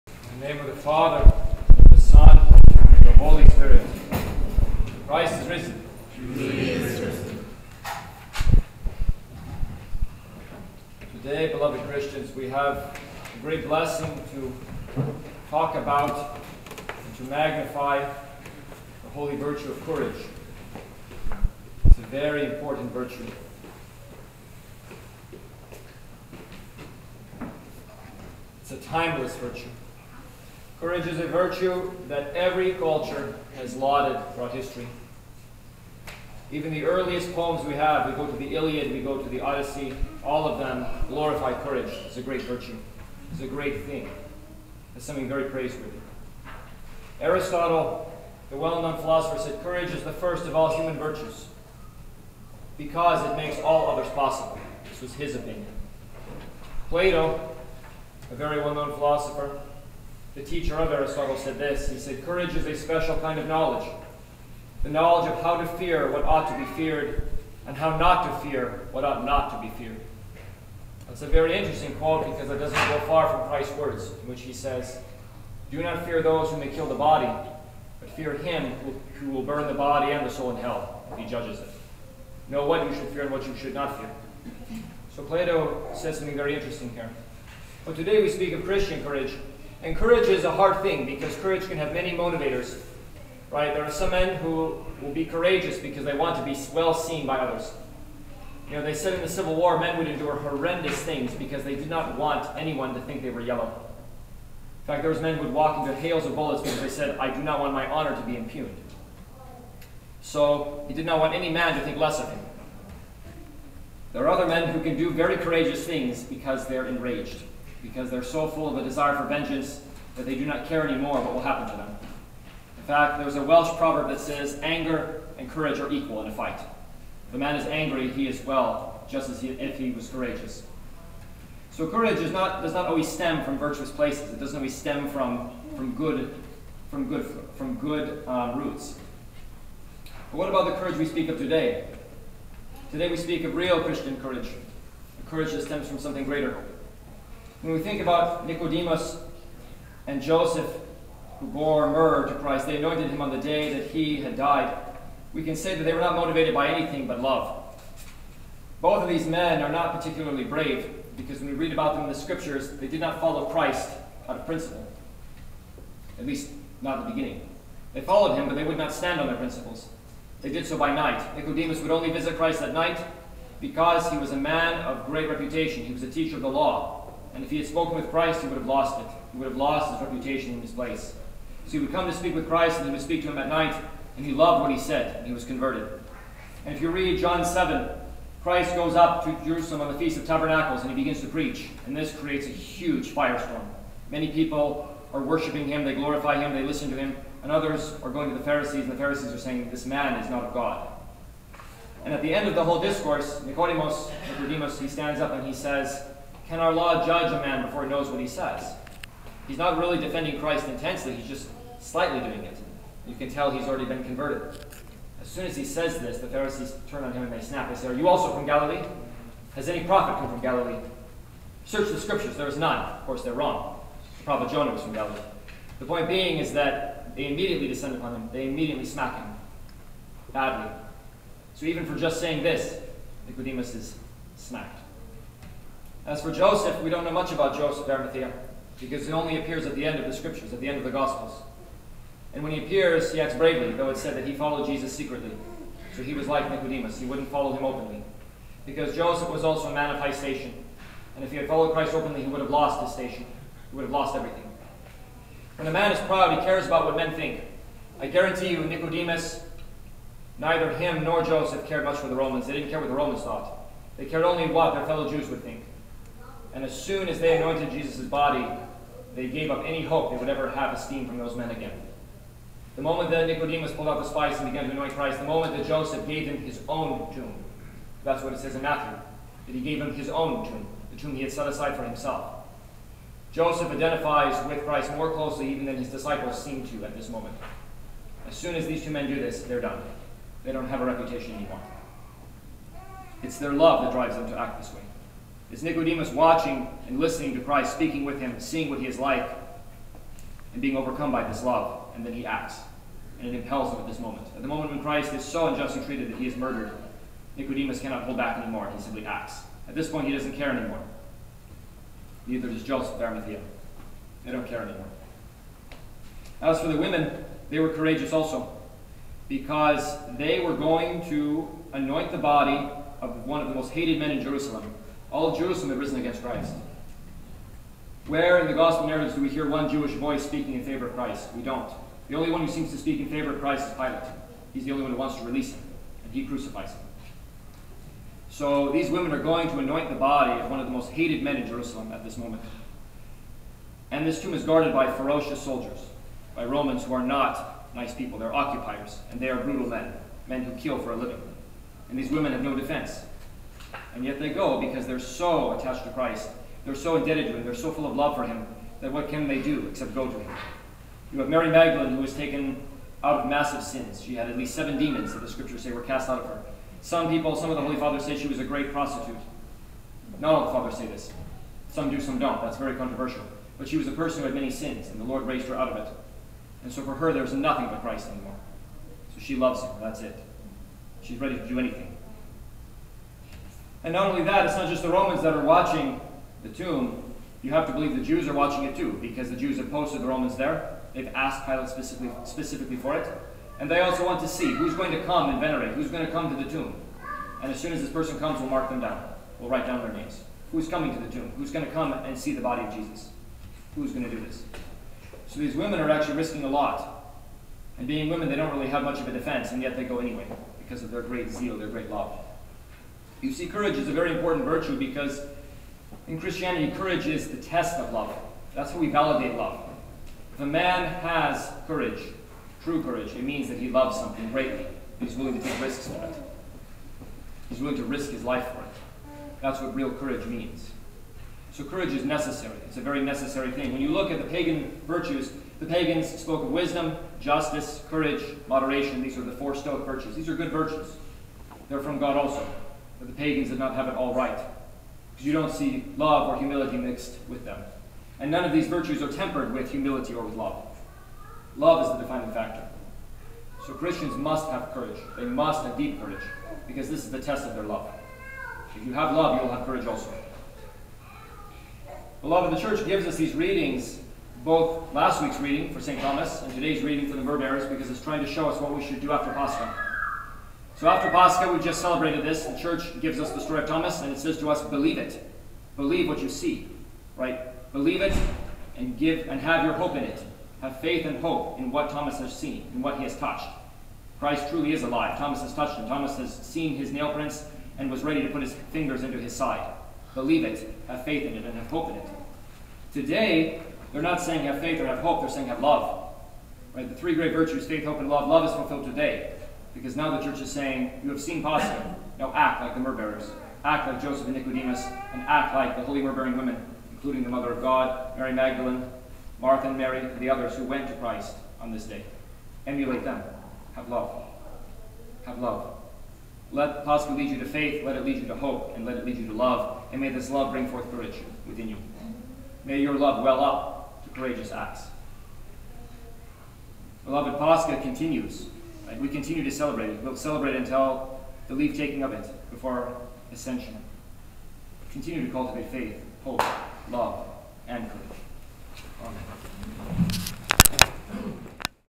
Fr John homily- Courage
Fr John homily on 4/16/21 at St John the Wonderworker in Eugene.